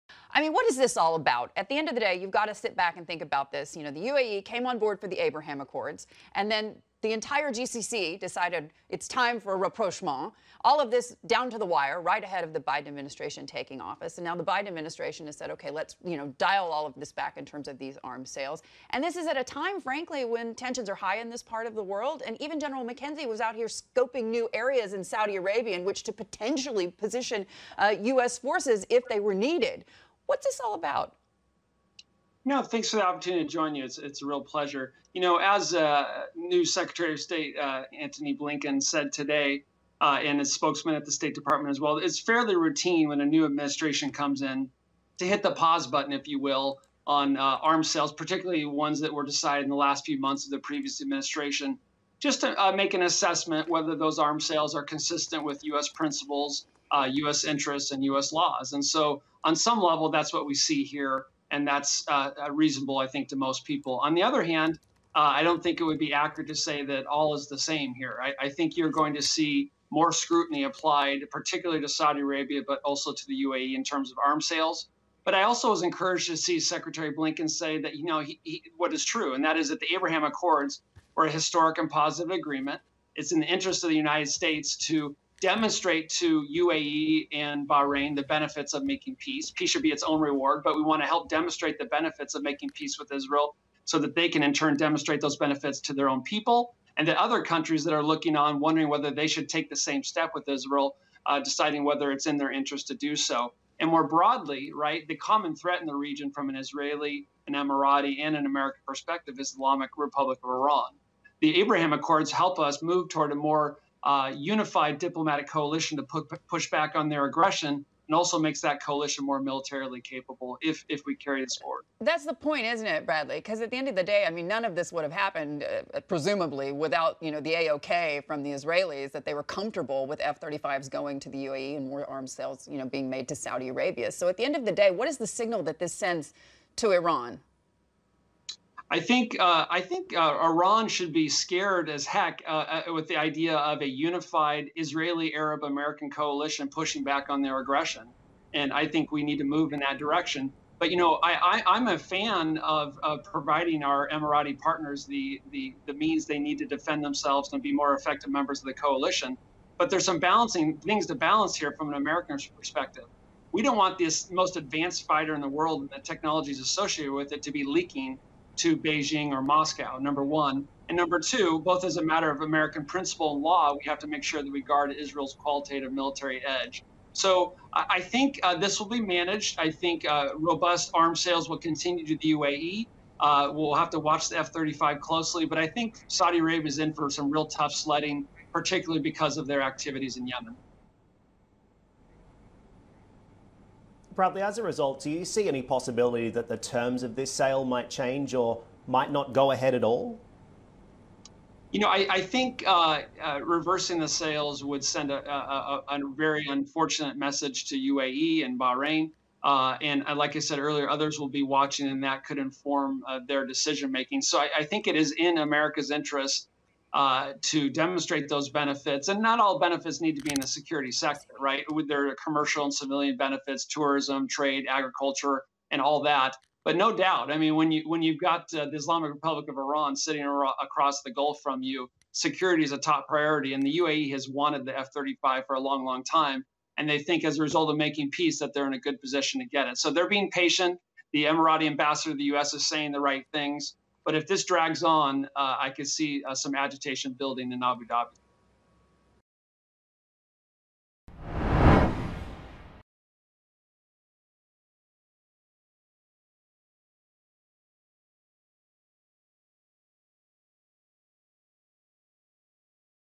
Reversing F-35 sales to the UAE, Saudi Arabia would send an unfortunate message: Director
The Foundation for Defense of Democracies' Bradley Bowman weighs in on President Biden's suspension of arms sales to Saudi Arabia and the UAE and what potential signal it sends to Iran.